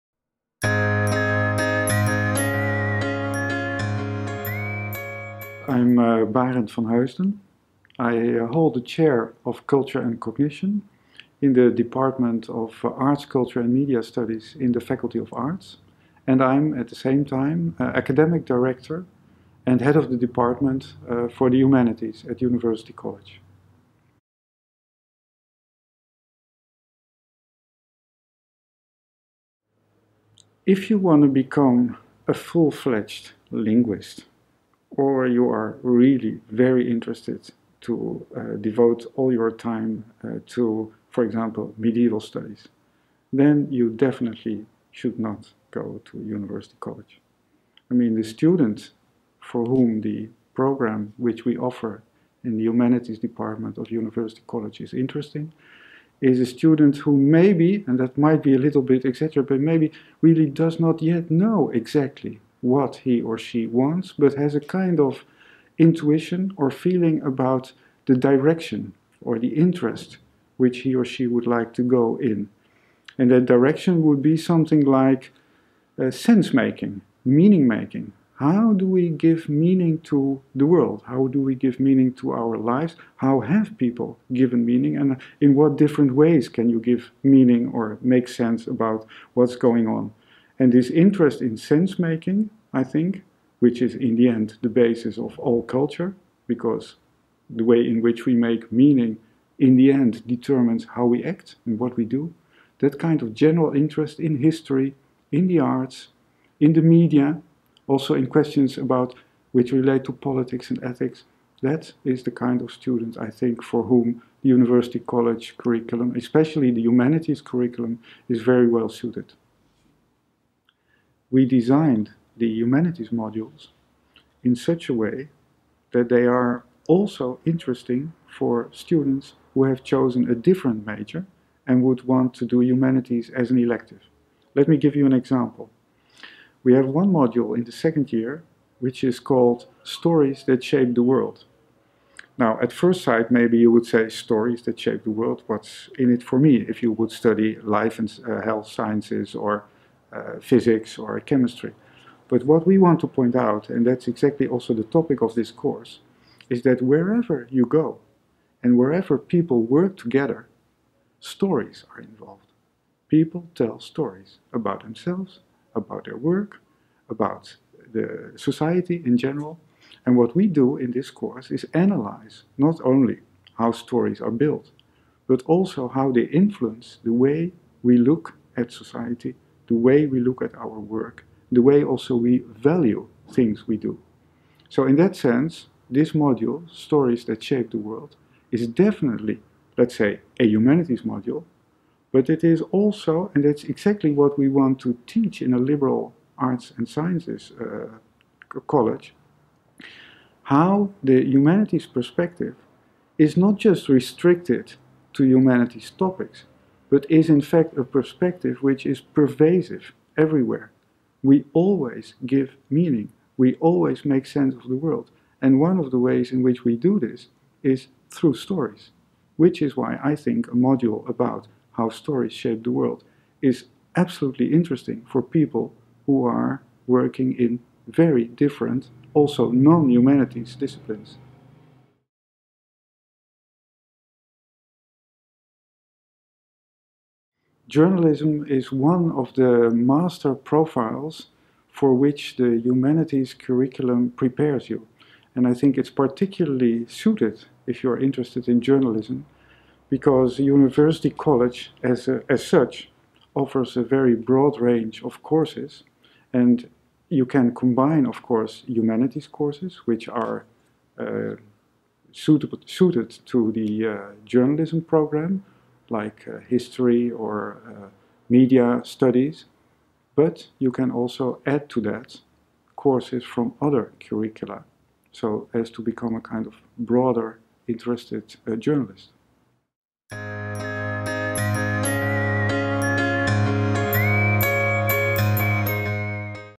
Journalism, Meaning-Making, and the Humanities Major at UCG
Prof. Dr. Barend van Heusden, head of the humanities department at UCG, talks about the revamped humanities major at UCG.

What kind of students might be interested in this major? Can you become a journalist after completing the major at UCG? And more in answered in this video.

Production: Dejan Duric
Interview: Frederika Cazemier